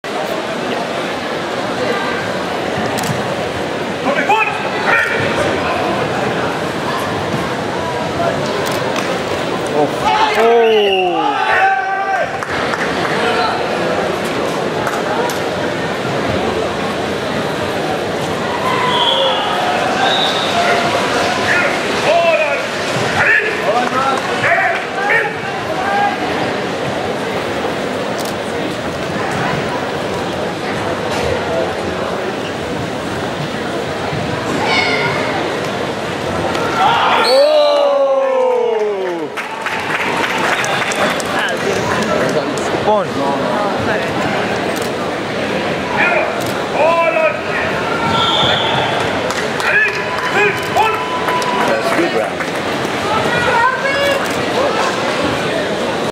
Gerard Felipe (AUS) vs ARGENTINA - Funakoshi Gichin Cup 14th JKA World Karate Championships
Gerard Felipe (AUS) vs (ARG) - 16-18 years old Male Kumite

14th Funakoshi Gichin Cup 14th JKA World Karate Championships
18-20th August, 2017 Limerick, Ireland